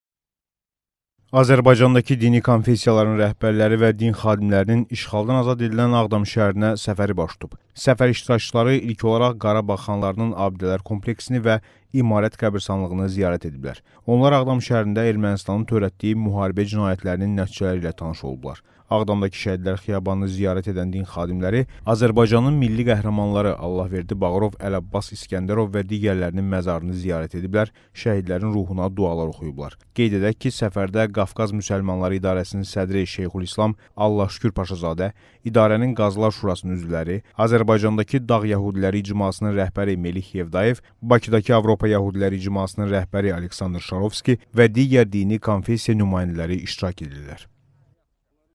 Din xadimlərinin Ağdam səfəri
Səfərdə Allahşükür Paşazadə ilə yanaşı dini konfessiya rəhbərləri də iştirak edir